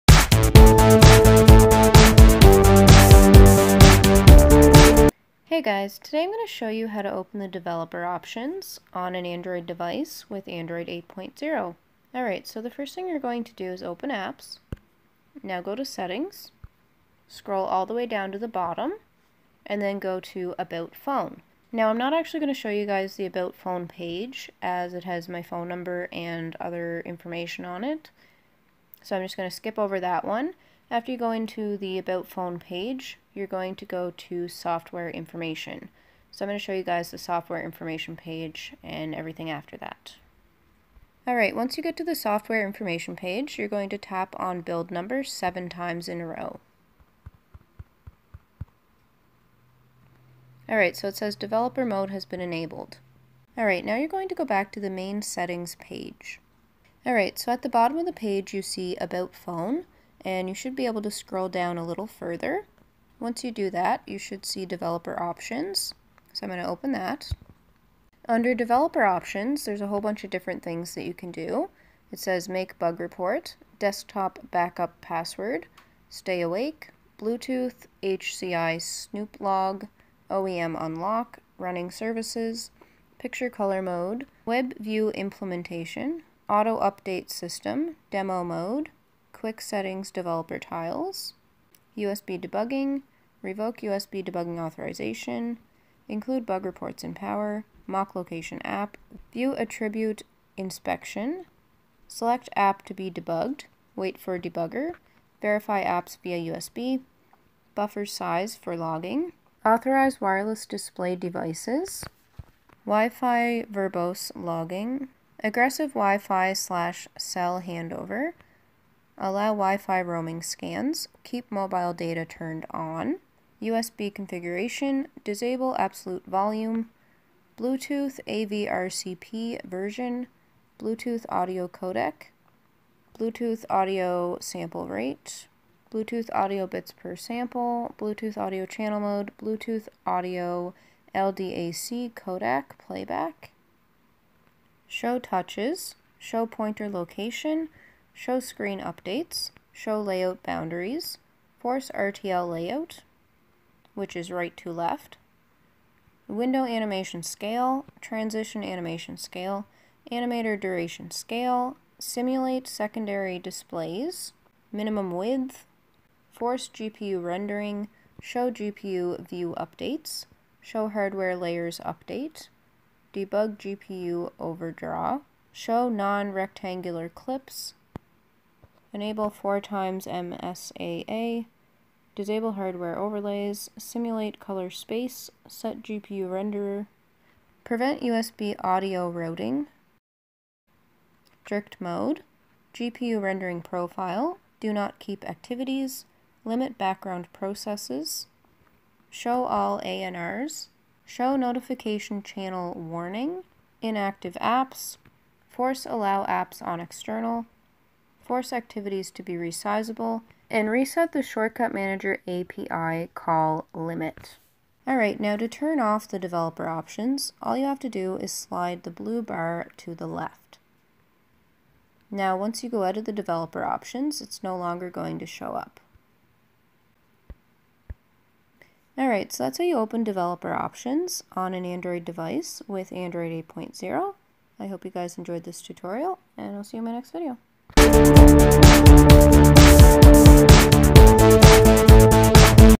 How To Open Developer Options on Android 8.0!!
This video will show you how to open the developer options on android 8.0

Thanks